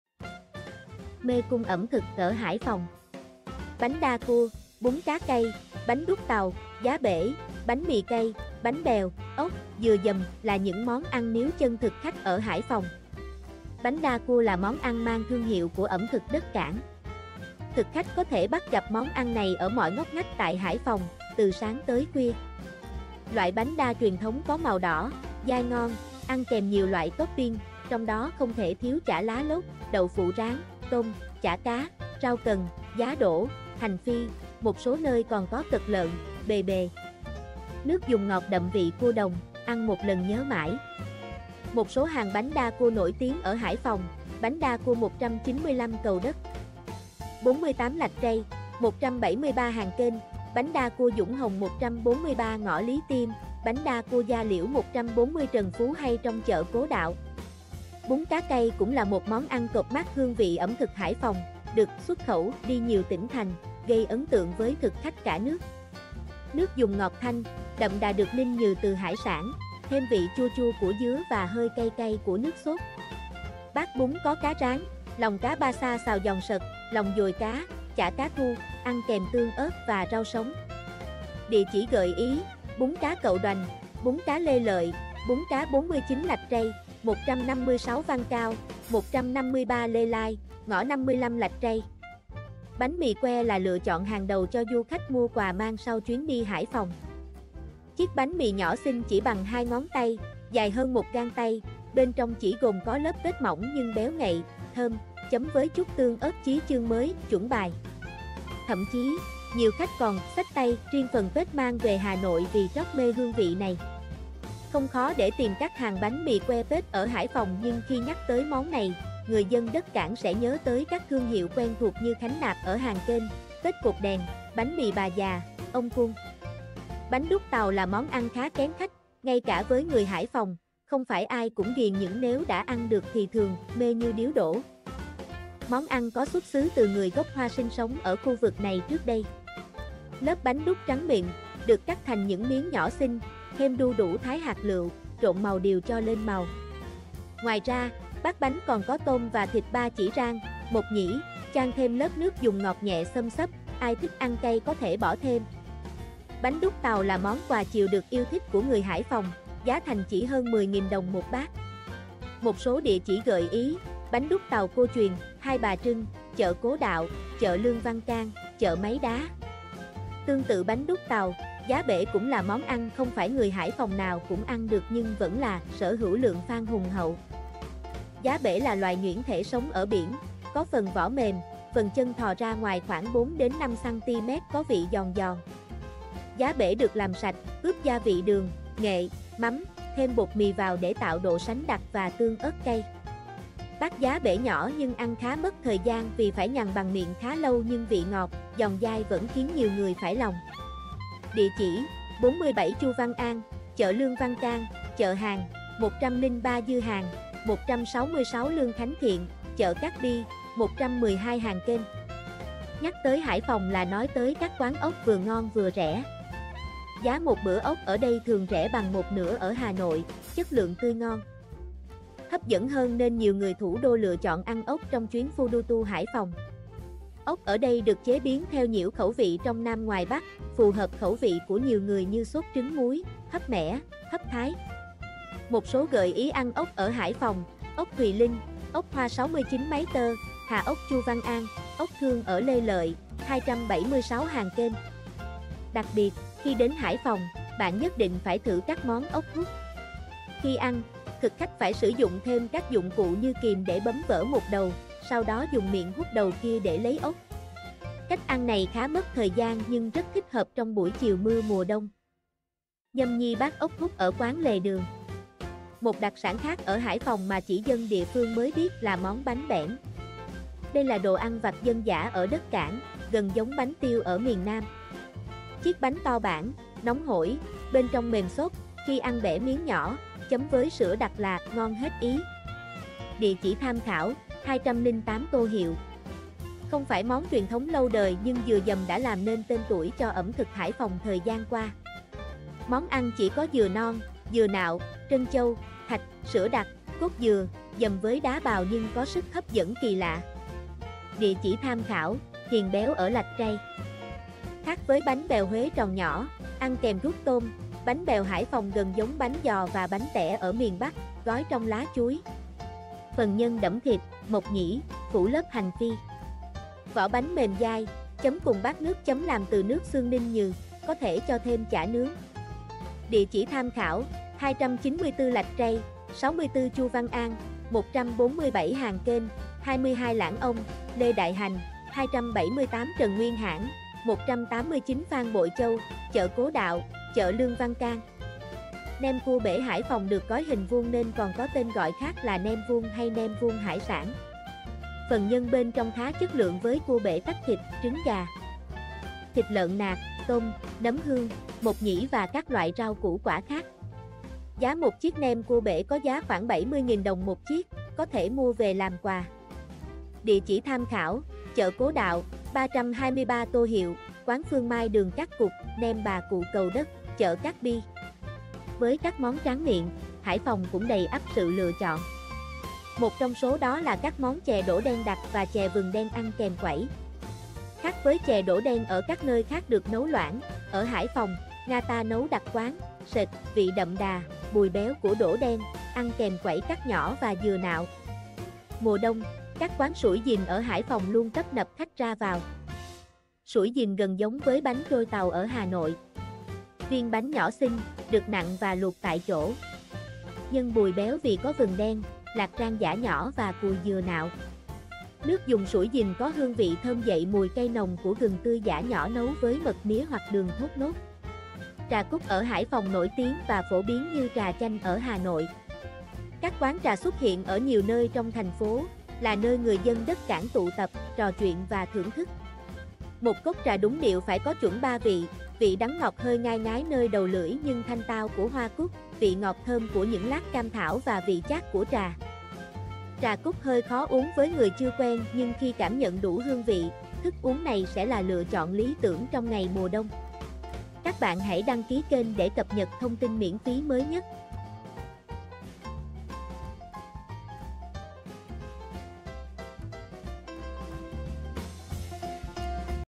'Mê cung ẩm thực' ở Hải Phòng
Bánh đa cua, bún cá cay, bánh đúc tàu, giá bể, bánh mì cay, bánh bèo, ốc, dừa dầm... là những món ăn níu chân thực khách ở Hải Phòng.

Bánh đa cua là món ăn mang thương hiệu của ẩm thực đất Cảng. Thực khách có thể bắt gặp món ăn này ở mọi ngóc ngách tại Hải Phòng, từ sáng tới khuya. Loại bánh đa truyền thống có màu đỏ, dai ngon, ăn kèm nhiều loại topping, trong đó không thể thiếu chả lá lốt, đậu phụ rán, tôm, chả cá, rau cần, giá đỗ, hành phi, một số nơi còn có cật lợn, bề bề. Nước dùng ngọt đậm vị cua đồng, ăn một lần nhớ mãi.

Một số hàng bánh đa cua nổi tiếng ở Hải Phòng: bánh đa cua 195 Cầu Đất, 48 Lạch Tray, 173 Hàng Kênh, bánh đa cua Dũng Hồng 143 ngõ Lý Tiêm, bánh đa cua Da Liễu 140 Trần Phú hay trong chợ Cố Đạo.

Bún cá cay cũng là một món ăn cộp mác hương vị ẩm thực Hải Phòng, được 'xuất khẩu' đi nhiều tỉnh thành, gây ấn tượng với thực khách cả nước. Nước dùng ngọt thanh, đậm đà được ninh nhừ từ hải sản, thêm vị chua chua của dứa và hơi cay cay của nước xốt. Bát bún có cá rán, lòng cá basa xào giòn sật, lòng dồi cá, chả cá thu, ăn kèm tương ớt và rau sống.

Địa chỉ gợi ý: bún cá cậu Đoành, bún cá Lê Lợi, bún cá 49 Lạch Tray, 156 Văn Cao, 153 Lê Lai, ngõ 55 Lạch Tray...

Bánh mì que là lựa chọn hàng đầu cho du khách mua quà mang sau chuyến đi Hải Phòng. Chiếc bánh mì nhỏ xinh chỉ bằng hai ngón tay, dài hơn một gang tay, bên trong chỉ gồm có lớp pate mỏng nhưng béo ngậy, thơm, chấm với chút tương ớt chí chương mới 'chuẩn bài'. Thậm chí, nhiều khách còn 'xách tay' riêng phần pate mang về Hà Nội vì trót mê hương vị này.

Không khó để tìm các hàng bánh mì que pate ở Hải Phòng nhưng khi nhắc tới món này, người dân đất Cảng sẽ nhớ tới các thương hiệu quen thuộc như Khánh Nạp ở Hàng Kênh, pate Cột Đèn, bánh mì Bà Già, Ông Cuông...

Bánh đúc tàu là món ăn khá kén khách, ngay cả với người Hải Phòng, không phải ai cũng ghiền những nếu đã ăn được thì thường 'mê như điếu đổ'. Món ăn có xuất xứ từ người gốc Hoa sinh sống ở khu vực này trước đây. Lớp bánh đúc trắng mịn, được cắt thành những miếng nhỏ xinh, thêm đu đủ thái hạt lựu, trộn màu điều cho lên màu. Ngoài ra, bát bánh còn có tôm và thịt ba chỉ rang, mộc nhĩ, chan thêm lớp nước dùng ngọt nhẹ xâm xấp, ai thích ăn cay có thể bỏ thêm. Bánh đúc tàu là món quà chiều được yêu thích của người Hải Phòng, giá thành chỉ hơn 10.000 đồng một bát.